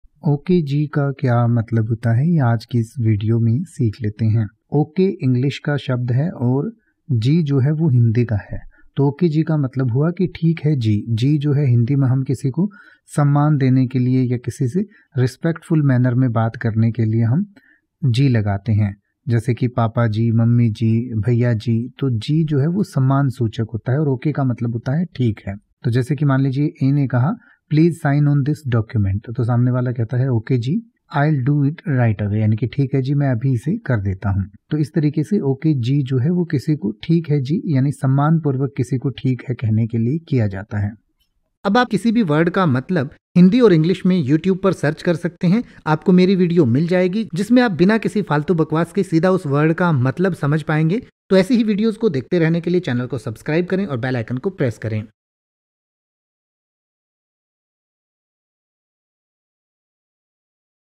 Ok ji meaning in Hindi | Ok ji ka matlab kya hota hai | English to hindi
इस वीडियो में आप Ok ji  का हिंदी में मतलब समझेंगे और साथ में Ok ji  का pronunciation भी सीखेंगें यानि की आप Ok ji  के अर्थ के साथ साथ ये भी सीखेंगें की Ok ji  को कैसे बोले या Ok ji  को बोलने का सही तरीका क्या है |

तो बस एक मिनट में सीखिए Ok ji  को|
Let’s learn Hindi Meaning of Ok ji  in detail.

Ok ji  का हिंदी में अर्थ होता है
ठीक है जी

""Ok ji"" is a Hindi expression that conveys agreement or acknowledgment in a respectful manner.

A: Please sign on this document.
B: Ok ji, I'll do it right away.

A: We'll need to leave early tomorrow morning.
B: Ok ji, I'll be ready on time.

Ok ji  को इन उदाहरणों से और अच्छे से समझ सकते हैं